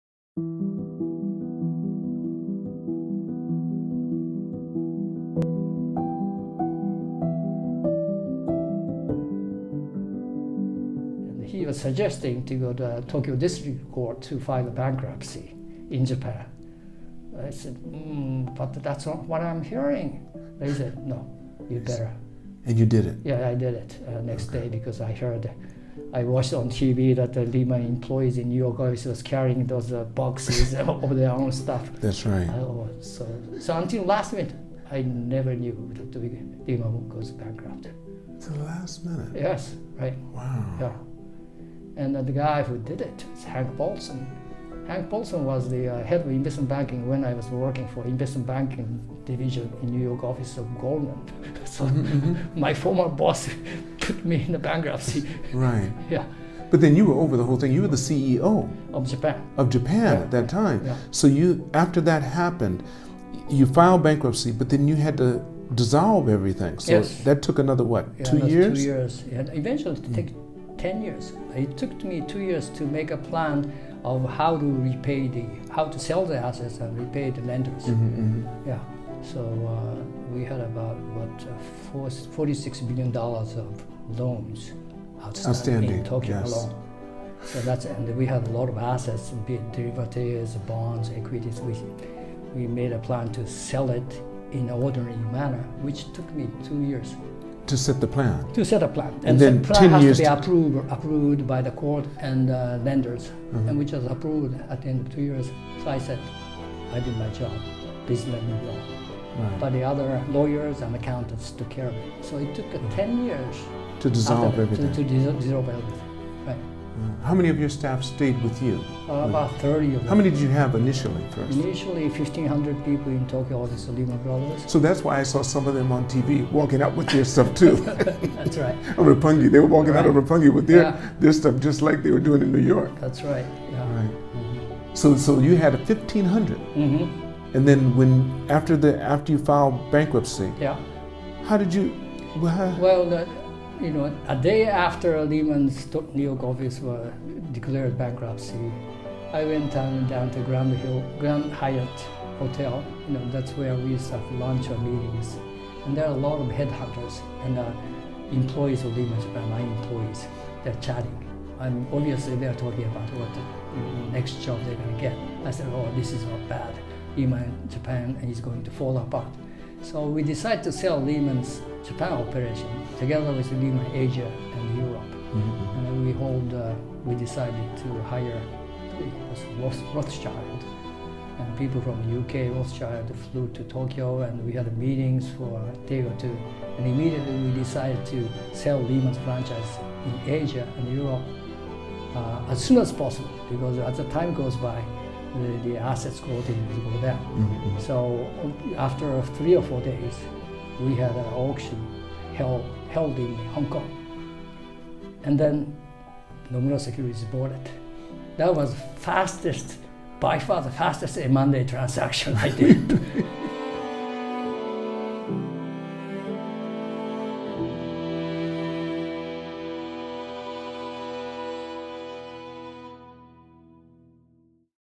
LEHMAN BROTHERS BANKRUPT JAPAN CEO SHOCKED Akio Katsuragi Lance E. Lee Podcast Highlight Episode #20
- What a great pleasure having Akio Katsuragi, founder/owner of Crosspoint Advisors, Inc., join me in a well-rounded and inspiring episode!  Born in 1952, Akio’s life juxtaposed with Japan’s rise as an economic giant is fascinating - from his Tokyo University education, further law studies in the United States, passing the bar in NY state, to his financial career at the IBJ (Industrial Bank of Japan) (now Mizuho bank), Goldman Sachs, Morgan Stanley, eventually becoming the CEO of Lehman Brothers in Japan.  He even shares a brief insight into how the Lehman Shock unfolded and his 10-year challenge to bring the ordeal to closure.  Akio explains how Crosspoint Advisors, Inc. works and its philanthropic aspirations.
His daily basketball workout, playing Yoko-bue (Japanese flute) and shamisen (Japanese string instrument), and six-year passion for kabuki dance keeps Akio motivated and energized! Enjoy!!